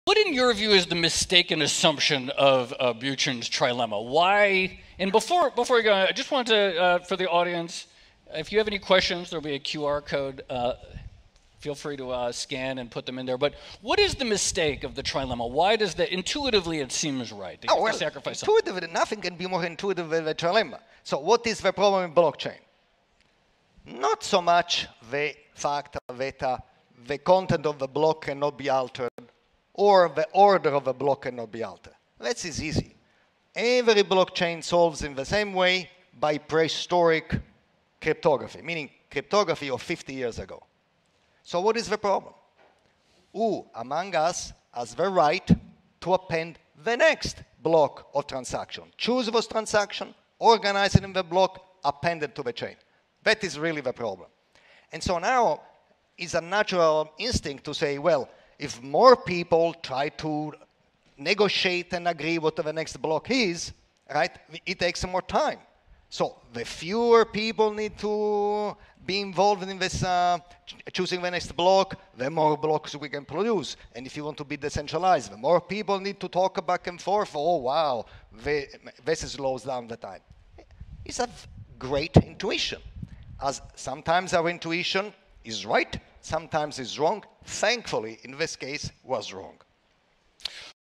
Why is Vitalik Buterin Wrong about the Trilemma? | Silvio Micali
Silvio Micali explains why the blockchain trilemma was a reasonable notion but misguided.